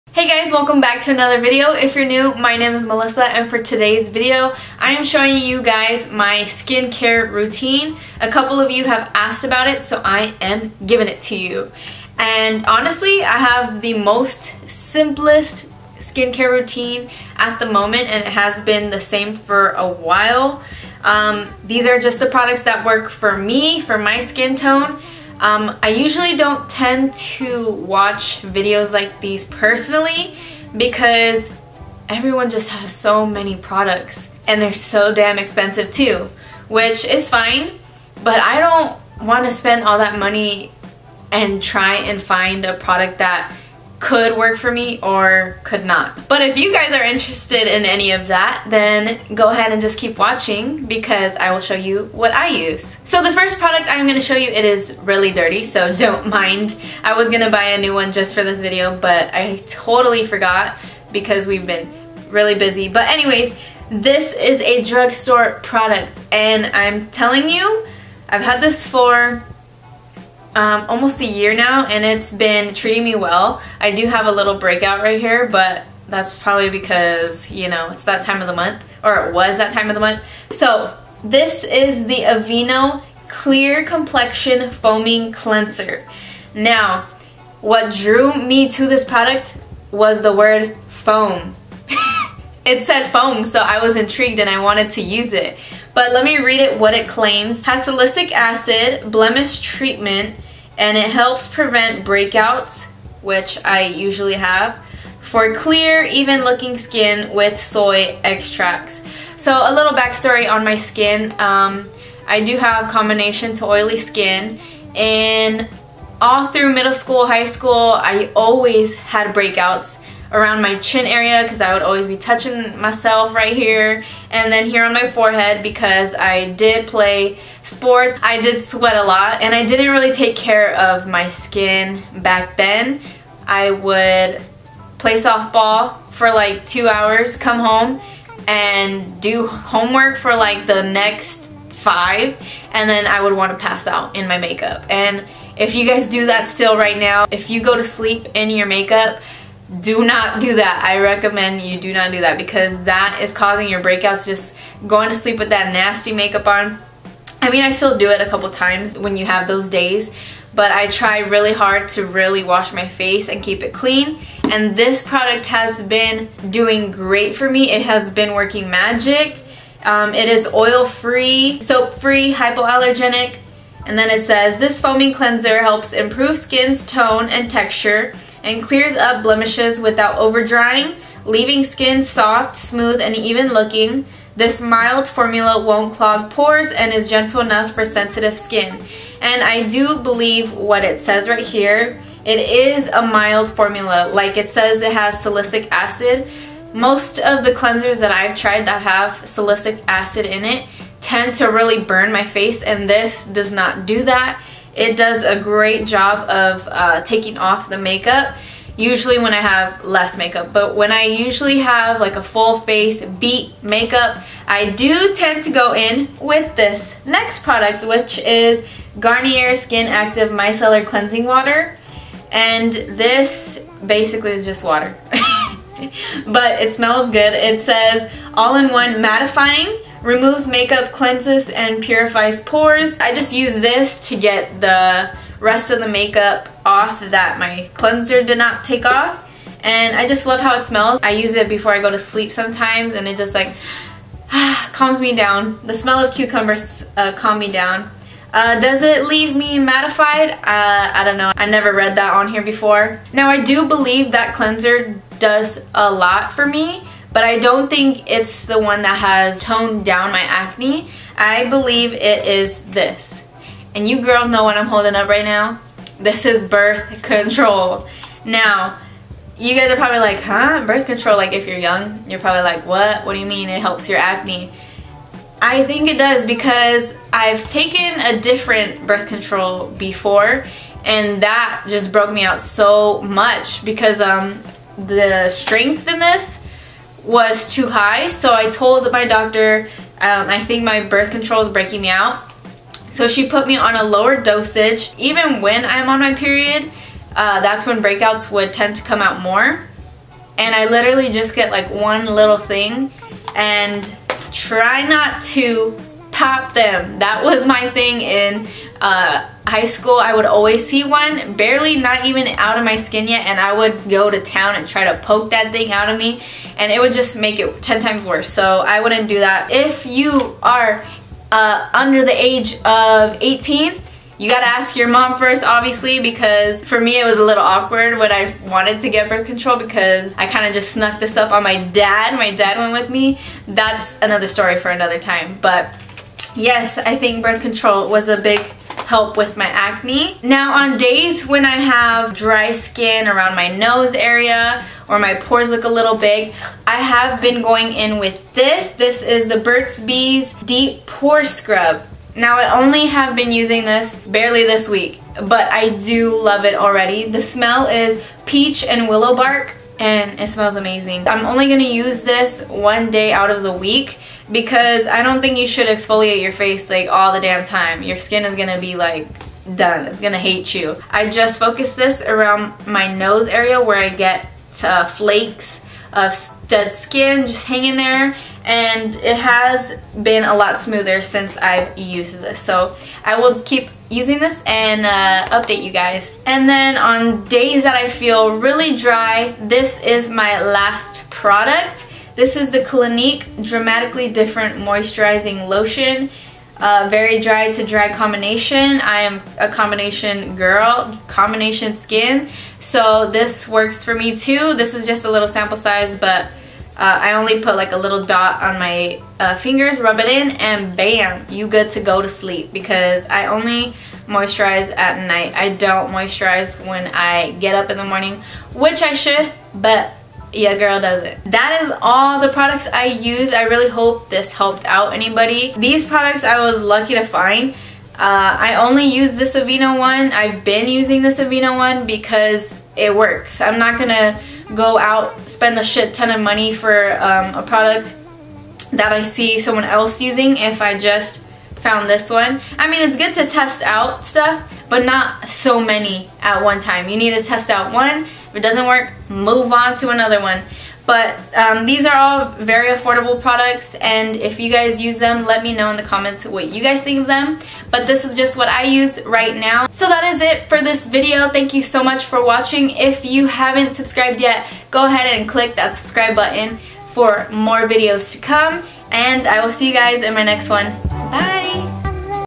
Skincare Routine 2018
These are the products I have been using at the moment and work for my skin! Hope this helps :)